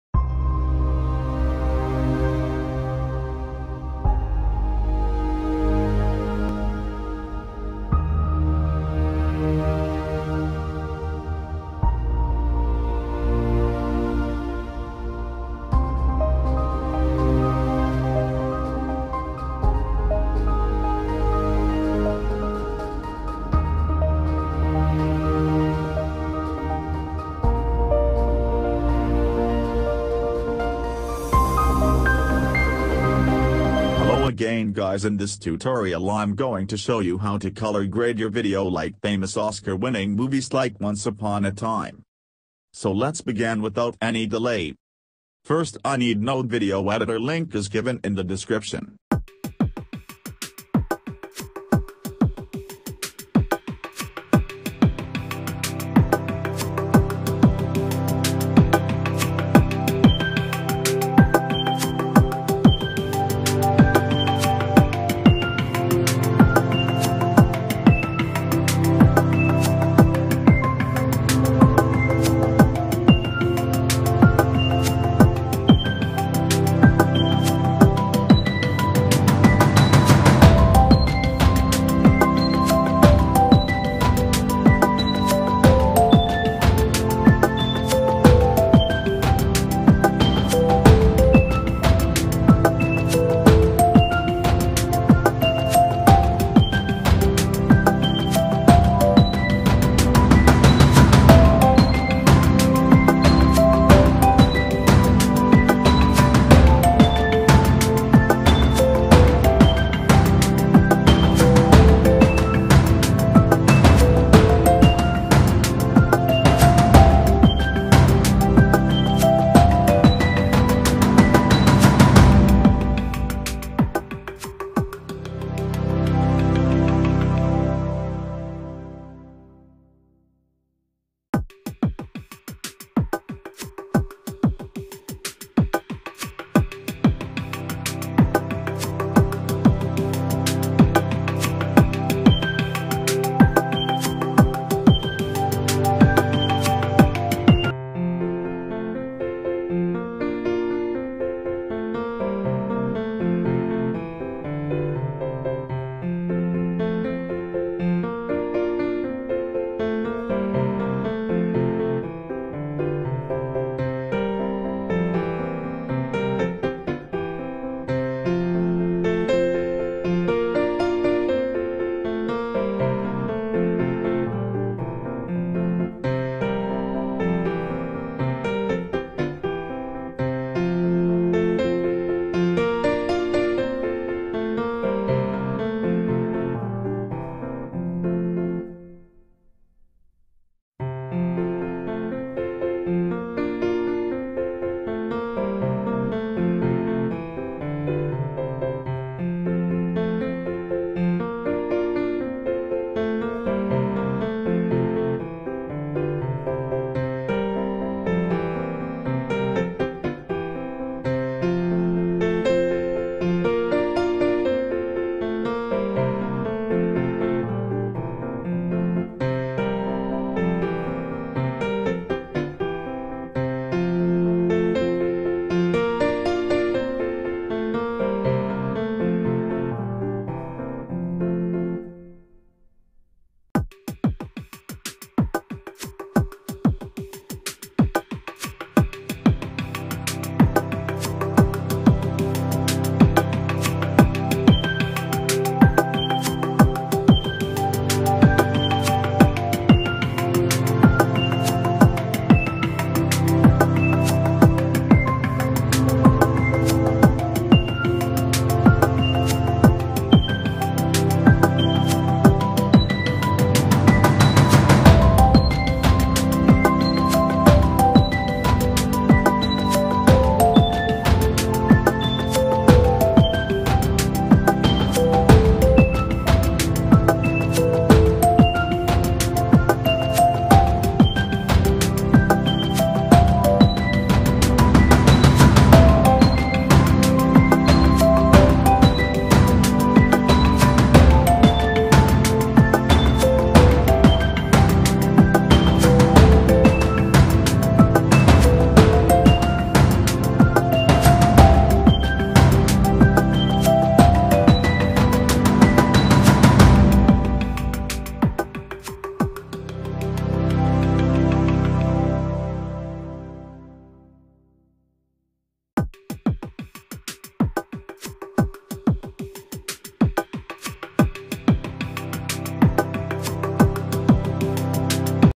COLOUR GRADING LIKE HOLLYWOOD MOVIE WITH MOBILE I NODE VIDEO
Colour grading your normal video to Oscar nomaniee movie like once upon a time, the joker etc. In node video application in very simple steps with your mobile phone.